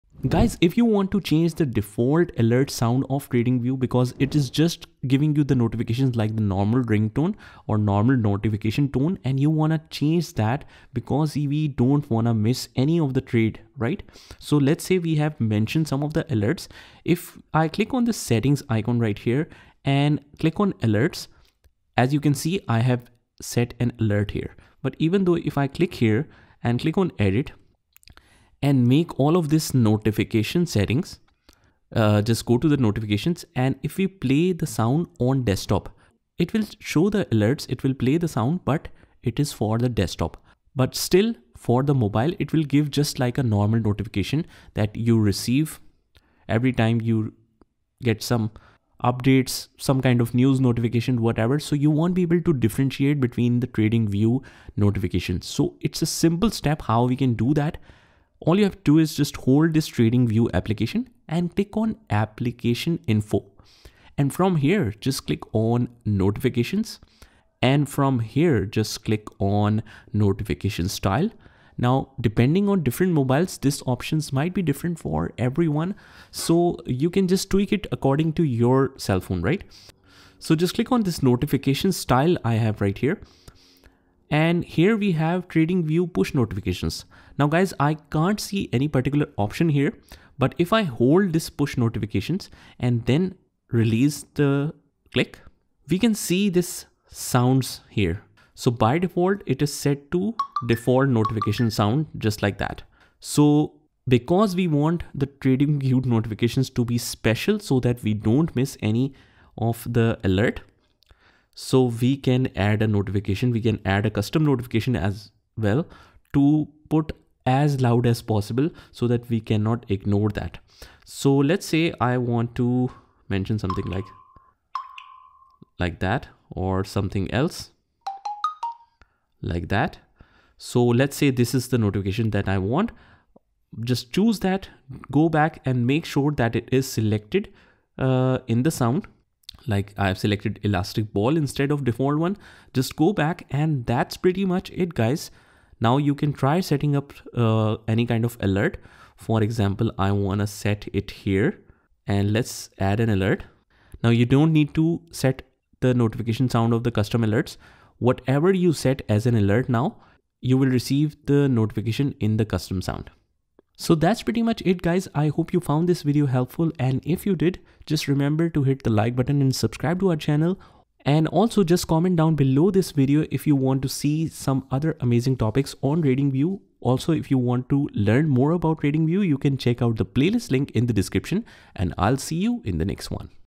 How to Change Default Alert Sound on TradingView Mobile App - 2025 | Step-by-Step Guide
Want to change the default alert sound on your TradingView mobile app? In this 2025 guide, I’ll show you how to customize your TradingView notifications and alerts for a better trading experience.

🔹 What You’ll Learn:
✅ How to access TradingView alert settings
✅ How to customize and change the default alert sound
✅ How to troubleshoot notification issues in TradingView
✅ Tips for optimizing TradingView alerts for trading efficiency

✔️ Customize alerts to match your trading style
✔️ Works for both Android and iOS users
✔️ Simple, step-by-step tutorial